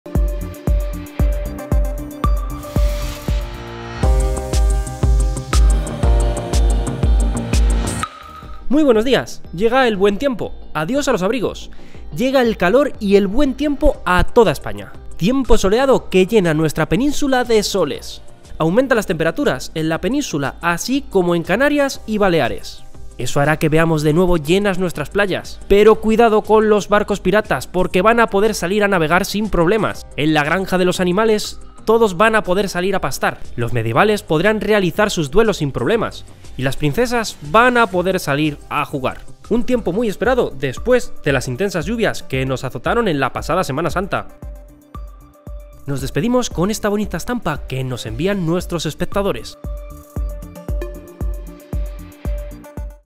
Tu Tiempo con Roberto Brasero versión click
Versión click del programa "Tu tiempo" de Roberto Brasero en Antena 3
Recreación aficionada del programa sin ánimo de lucro.